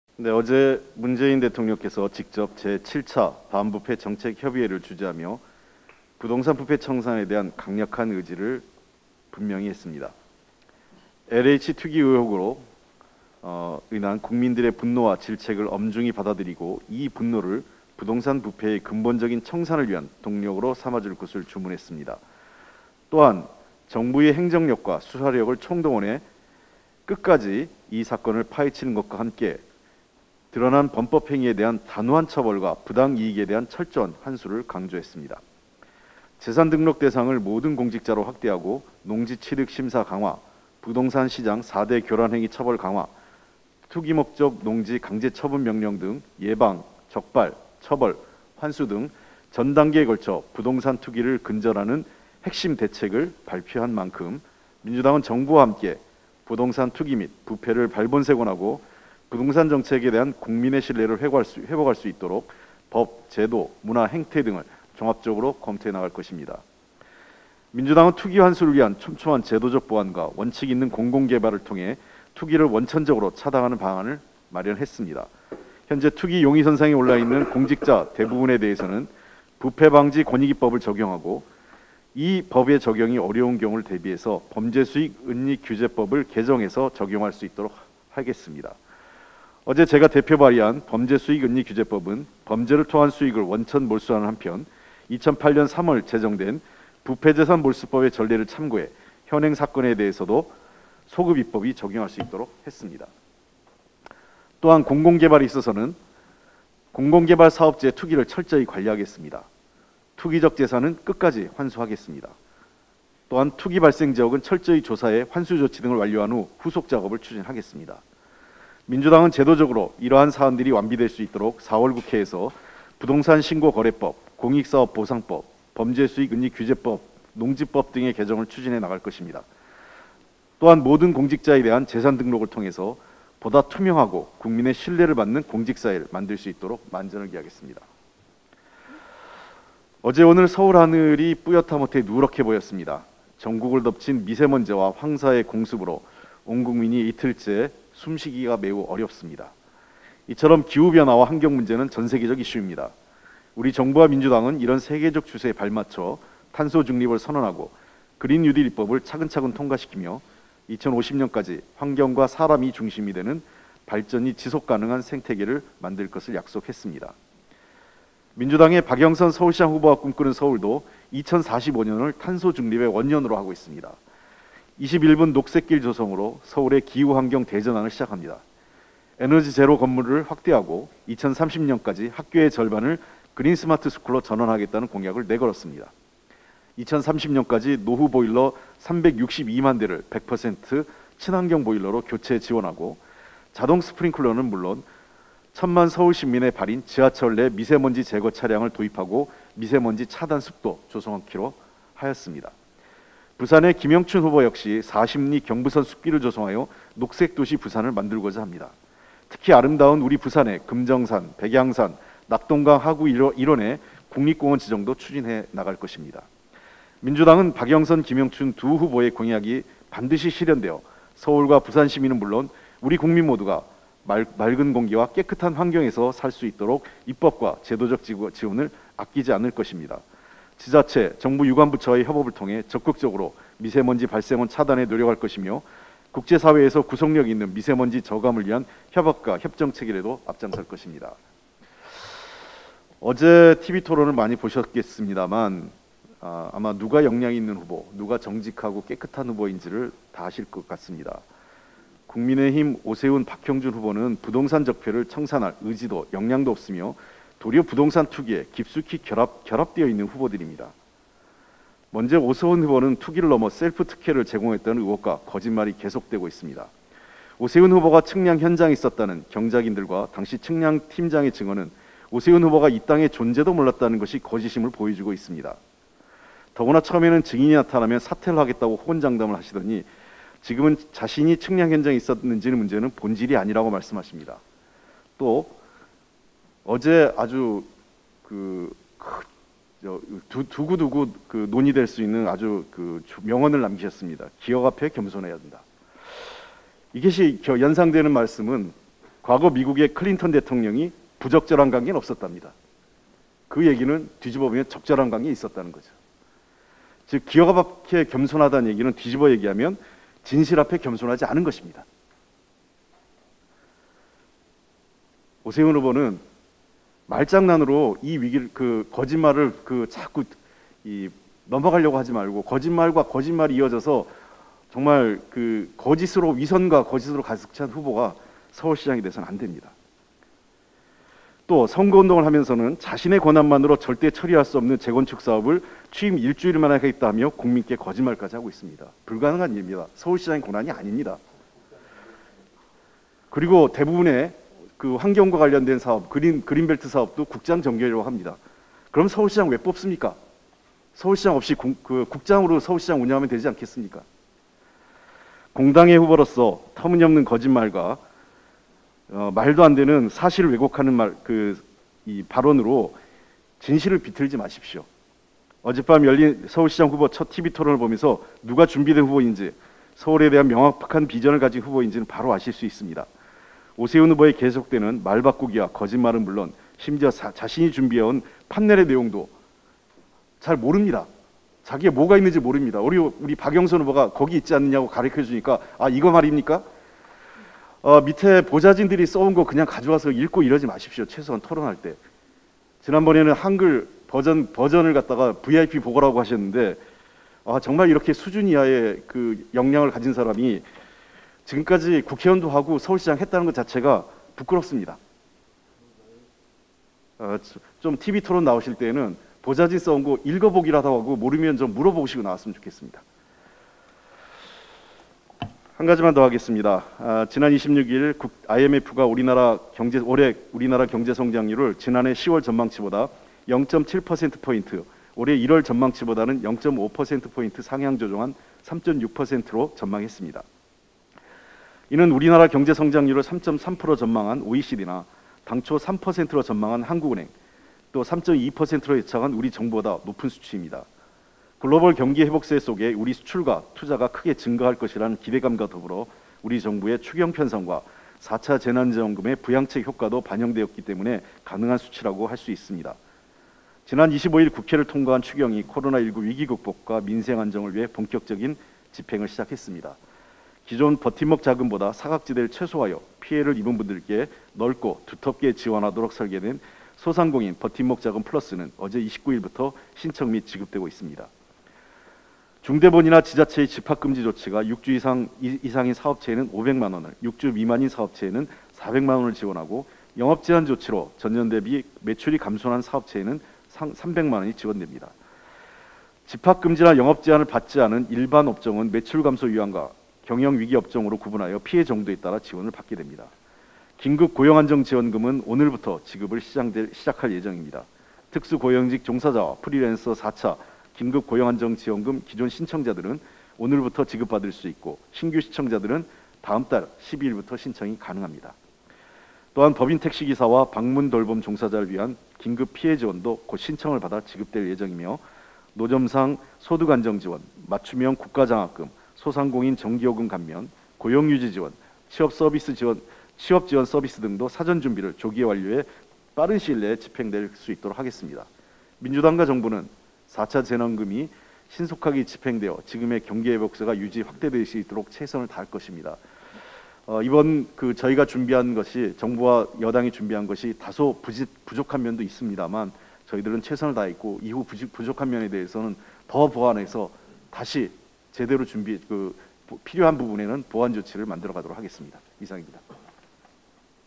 홍익표 "투기적 재산 끝까지 환수"
홍익표 더불어민주당 정책위의장은 30일 오전 당 원내대책회의에서 부동산 부패 청산을 강조했다.

(영상제공 : 더불어민주당, 영상편집 : 최윤지 PD)